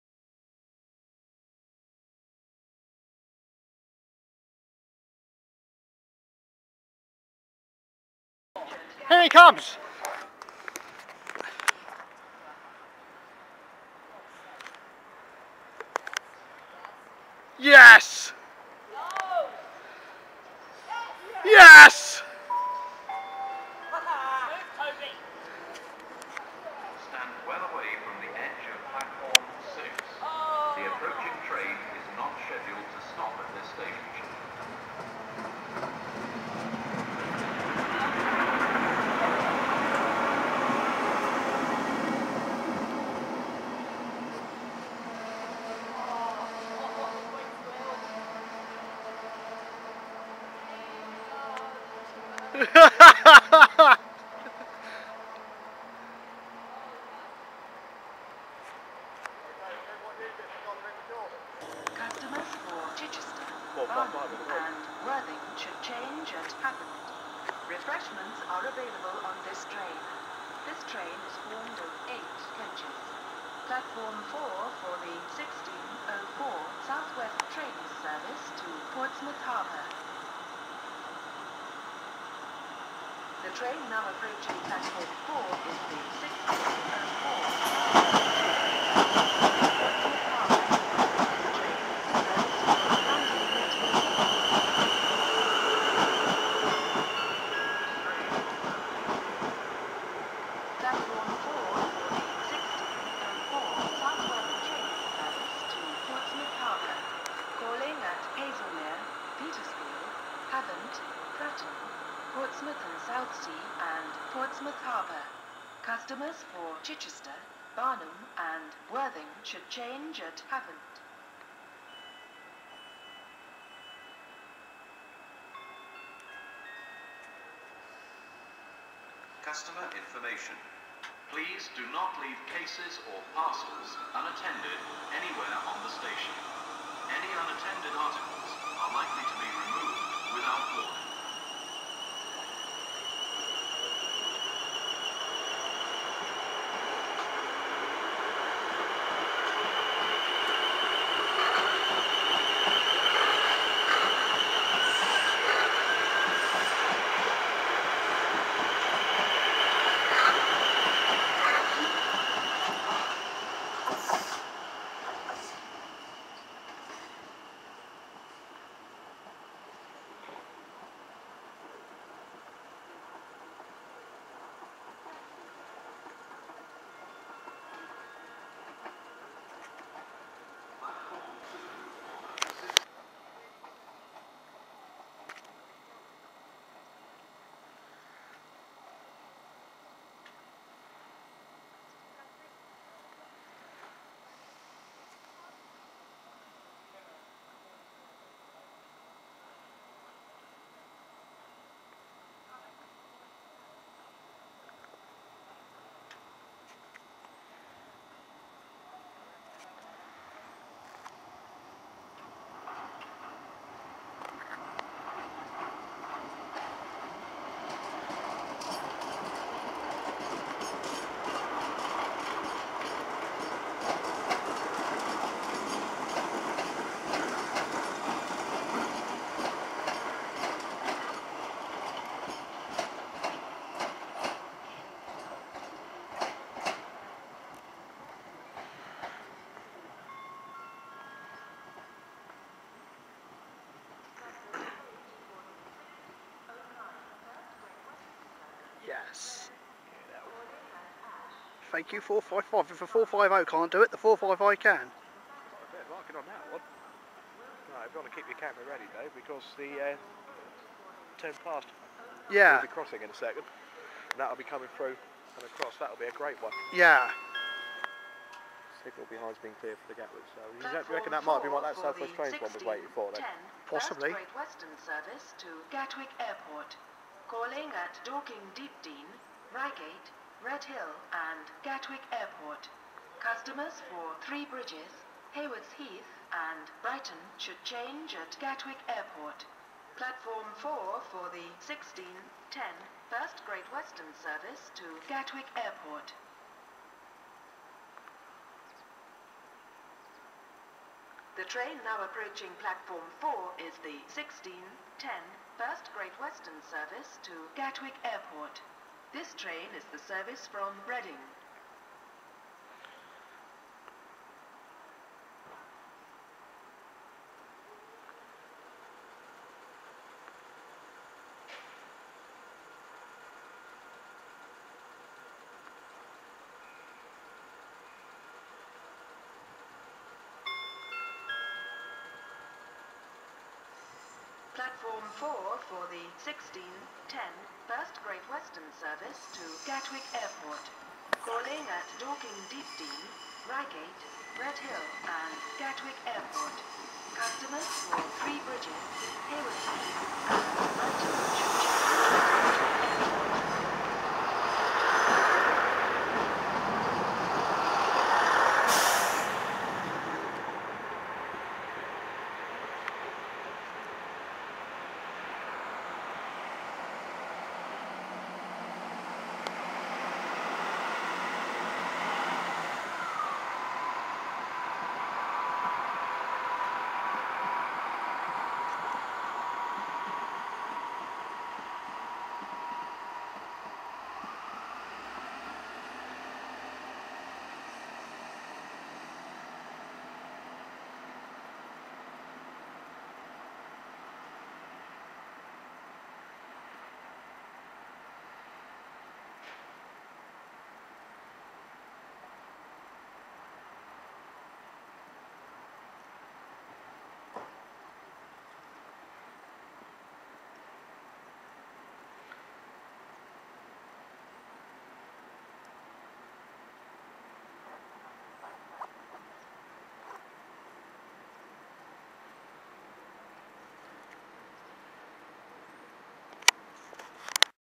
Guildford Station 13/11/2010
Some footage shot at Guildford on the 13th November 2010 including my first MPV leaf buster (well on film) (I forgot to hit record when one passed Westenhanger earlier in the year) 455, 450 & 166 Also in the carriage sidings where 1 x 458, 1x 455, 1 x 458 with the 455 sandwiched in between the junipers.